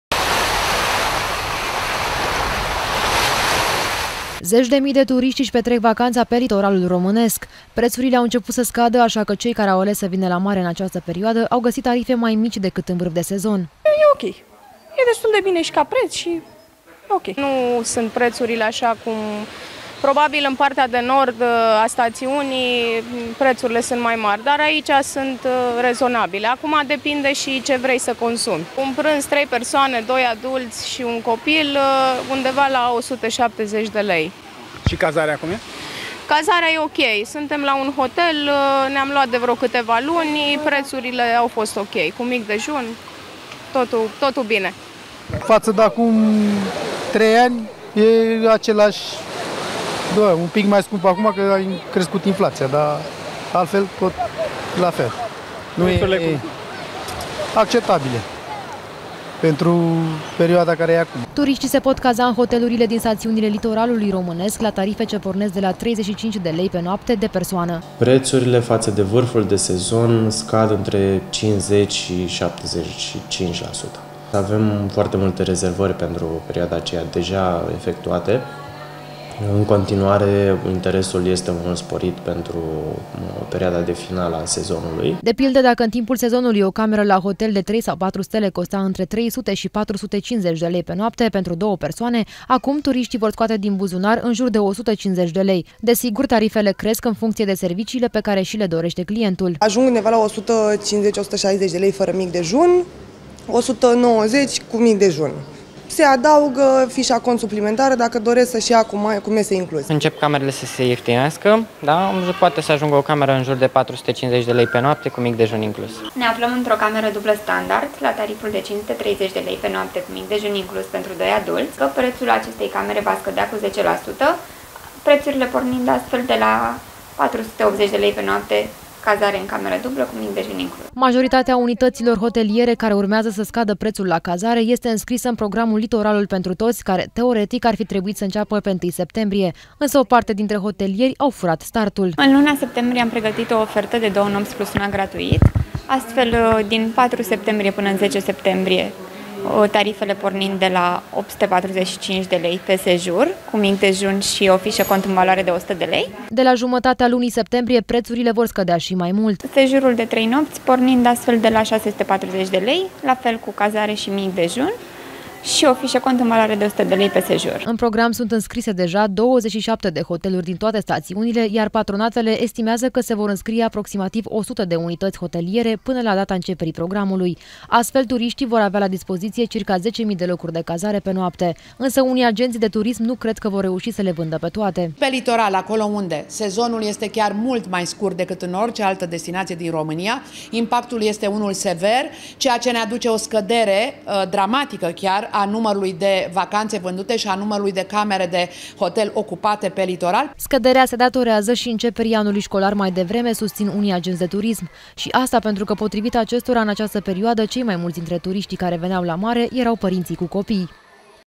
Vacanțe mai ieftine pe litoral
Veşti bune pt cei care n-au ajuns încă la mare. Prețurile de pe litoralul românesc vor începe să scadă în perioada imediat următoare. Tarifele vor fi astfel reduse cu până la 70%. În doar câteva zile va începe și programul „Litoralul pentru Toți”. Mulți hotelieri au furat startul așa că au ieftin deja cazarea. De pildă, dacă până acum pentru o noapte la un hotel de trei stele plăteați 300 de lei, acum aceasta va costa 80 de lei.